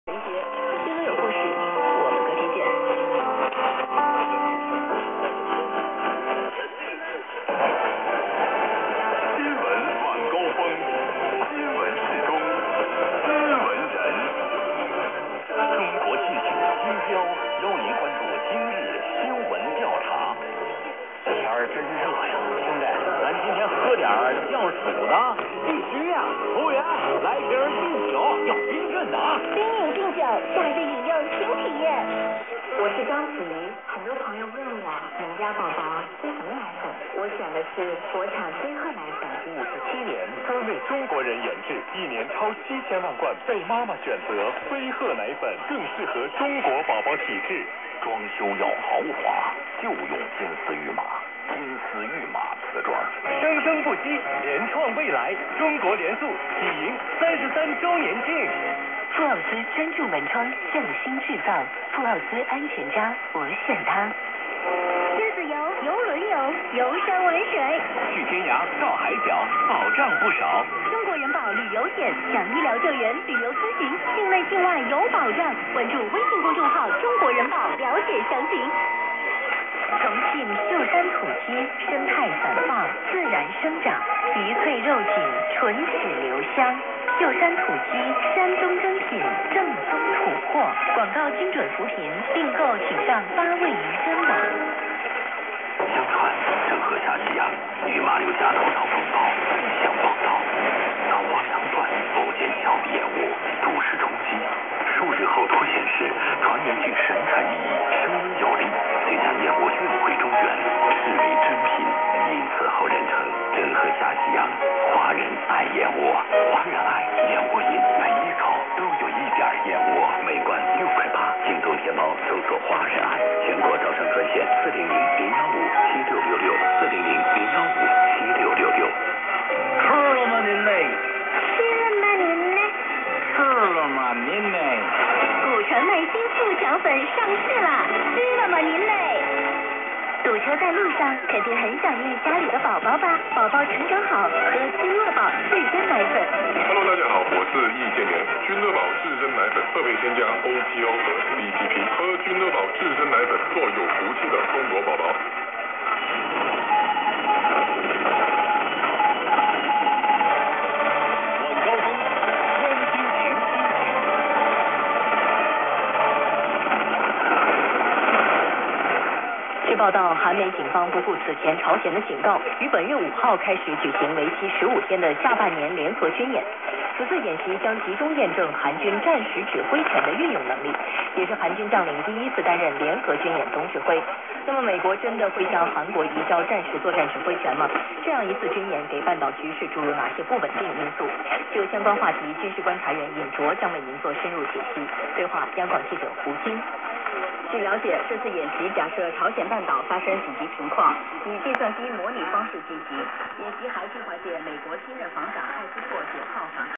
CNR1 1098 kHz
ID as  "Zhongguo zhi Sheng" (Voice of China)
1000 kW Golmud, P.R.C.
QTH: Middle Volga region of Russia
2019/08/07